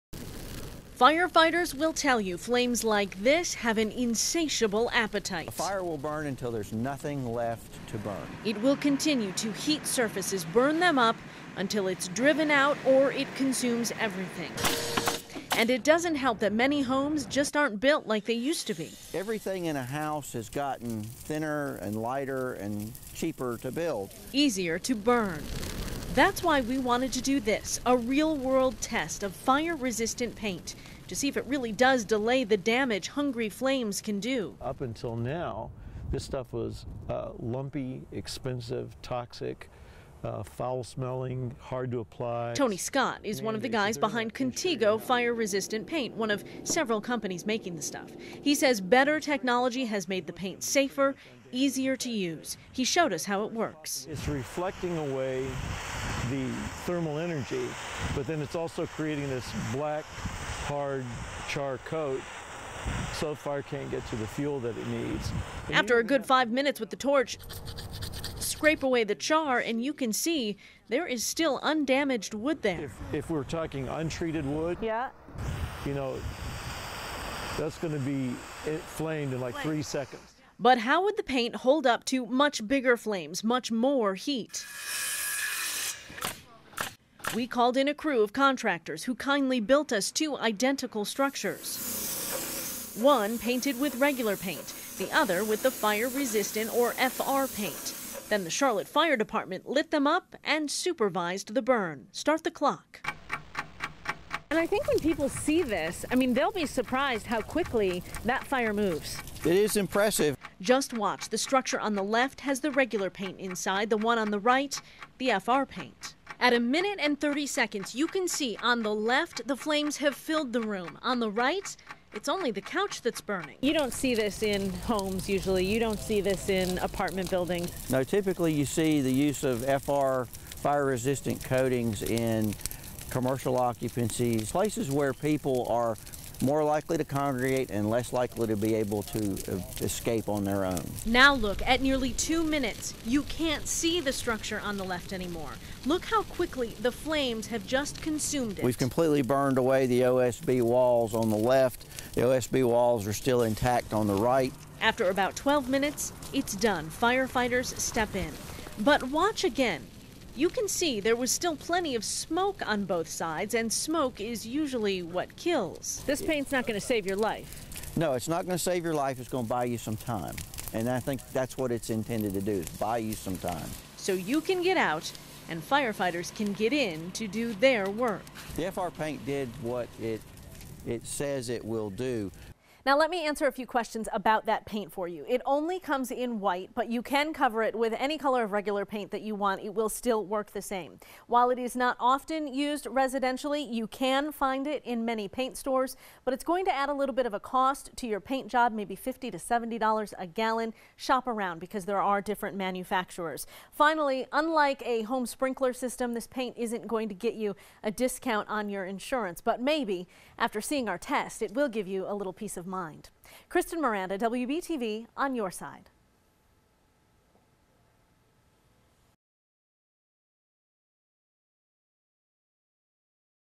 Fire Resistant Paint Investigation
Homes are not built like they used to be.  Materials are lighter and therefore, more flammable.  This investigation, done by WBTV, shows a side by side burn test of a structure coated in intumescent paint with an untreated structure.